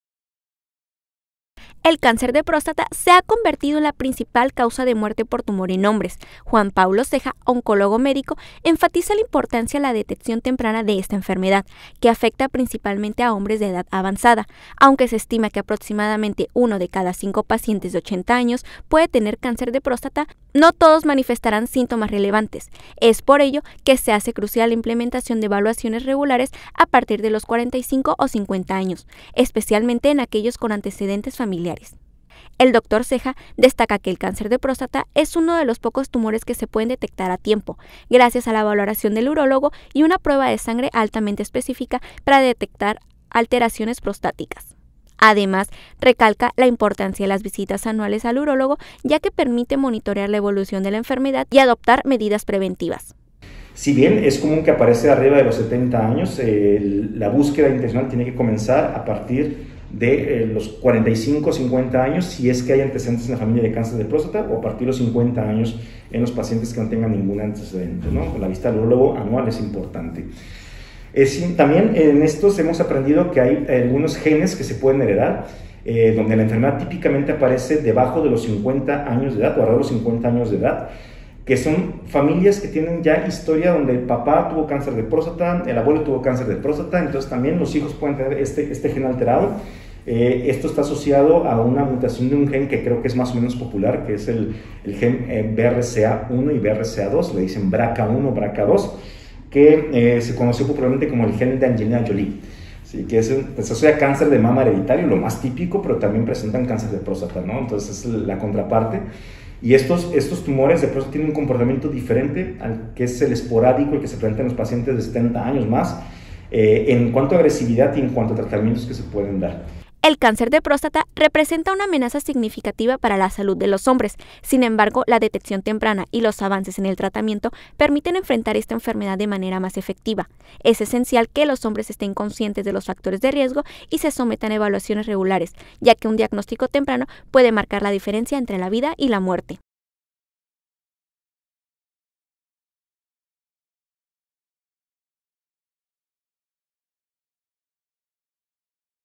Oncólogo asegura que el cáncer de próstata es la principal causa de muerte en hombres.👨‍⚕️☝️
LaNota con Itzel Rocha